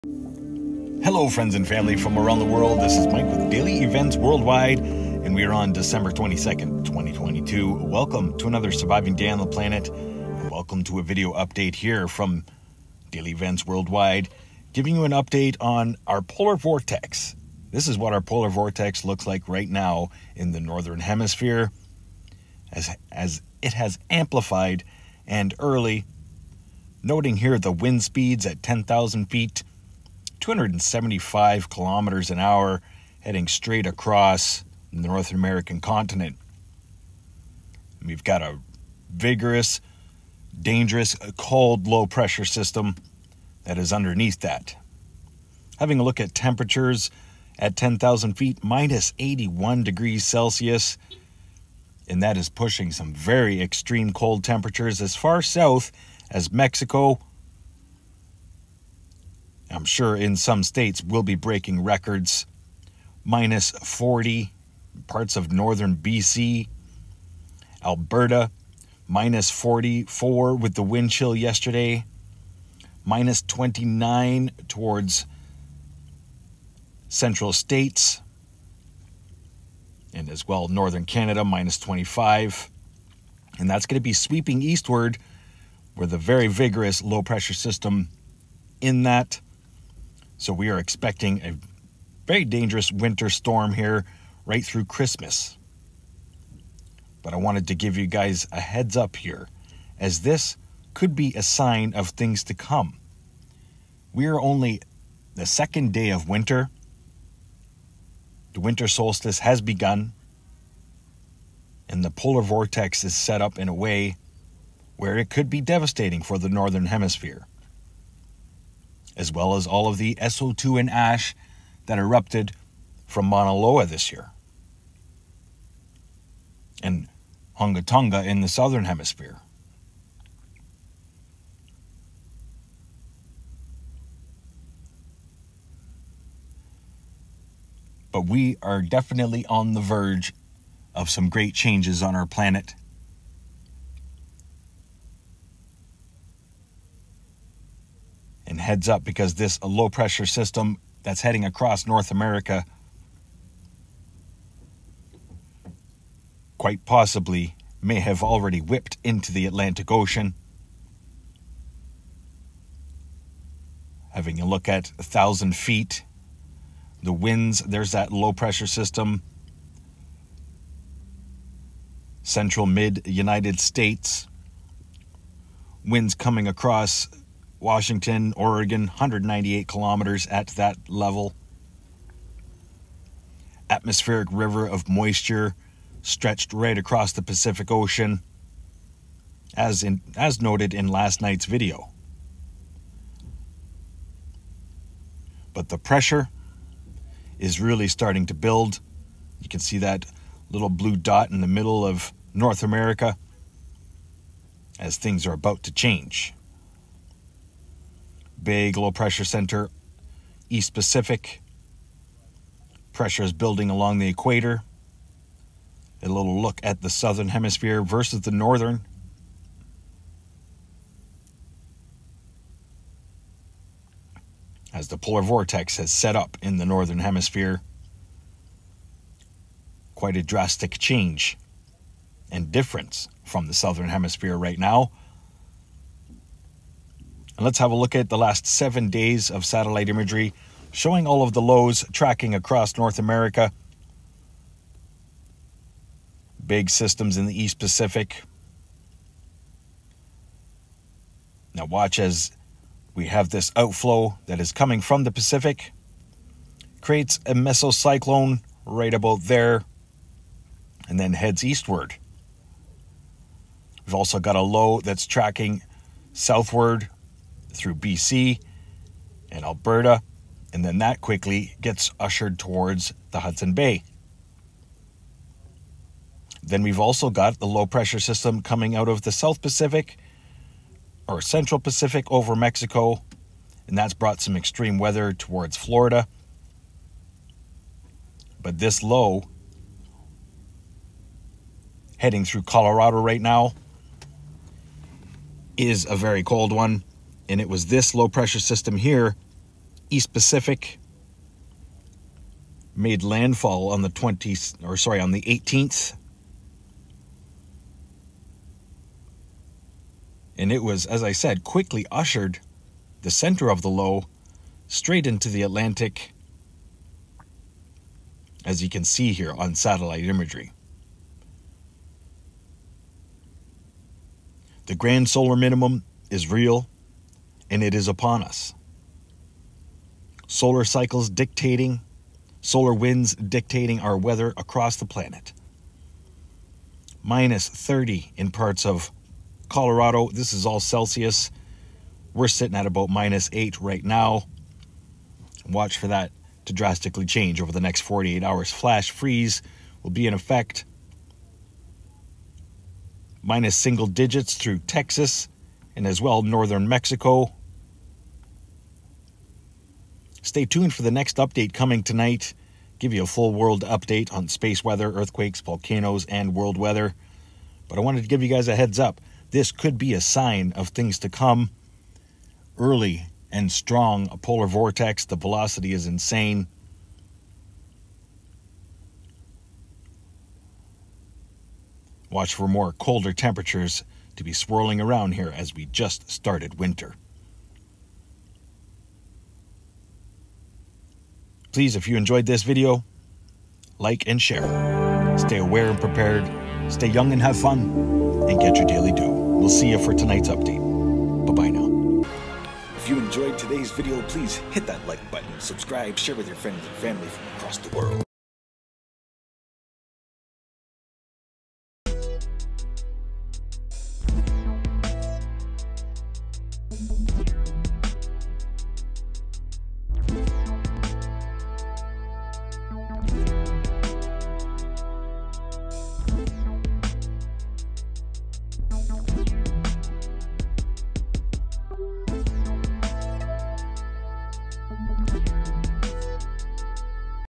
The Day After Tomorrow… A SIGN Of Things To Come??
POLAR VORTEX 2022 / U.S & Canada EXTREME COLD / A SIGN Of Things To Come??
December 22 2022